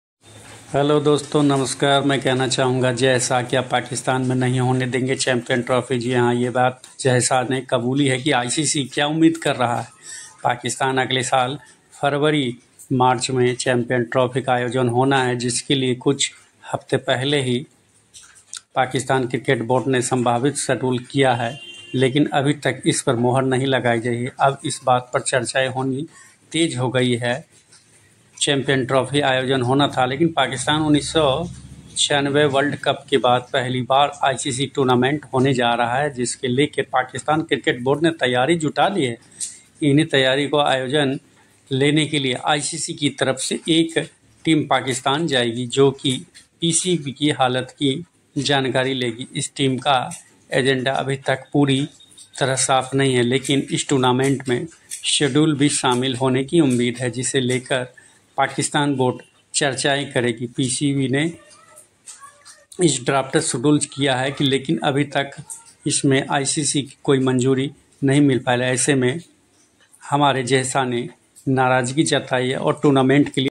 Why Pakistan HATES Jay Shah
जय शाह क्या पाकिस्तान में नहीं होने देंगे चैंपियंस ट्रॉफी, ICC से क्या उम्मीद कर रहा PCB?cricket